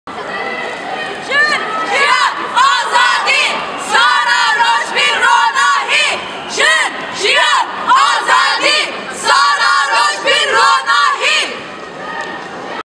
PARIS MASSACRE DEMONSTRATION
On 9 January 2013 an atrocious crime was committed in the center of Paris which has deeply shaken all Kurdish women's organizations and the Kurdish people and international women's organizations. . Three Kurdish women politicians and fighters for freedom and women's liberation - Sakine Cansız, Fidan Dogan and Leyla Şaylemez - were brutally and dastardly murdered.

This cruel crime in central Paris concerns all people fighting for freedom, justice, women's rights and democracy. Women, peoples and progressive political forces from all over the world are still calling for an immediate clarification where assassins still haven't been bought to justice.

It has been a year since the brutal massacre of the 3 Kurdish women in Paris!!
This is not just a blow for Kurdish people and Kurdish women but all women!!

Dont Stay Silent !!

Se Jinen Azad
Jin Jiyan Azadi